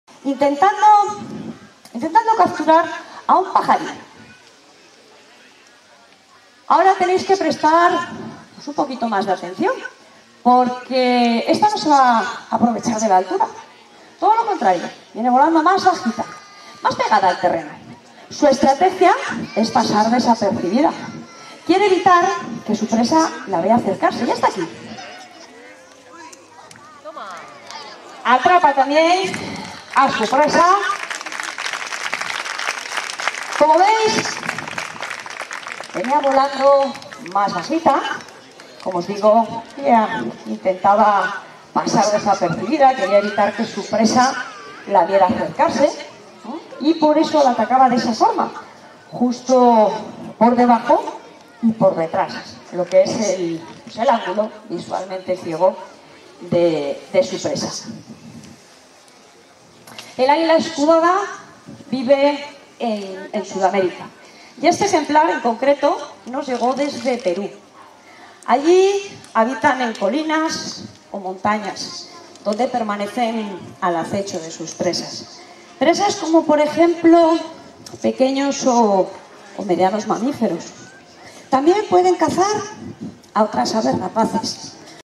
🦅❤️🌳Parque de la Naturaleza de Cabárceno Exhibición de vuelo de aves rapaces🌳🦅🦅(2)
🦅❤️🌳Parque de la Naturaleza de Cabárceno Exhibición de vuelo de aves rapaces🌳🦅🦅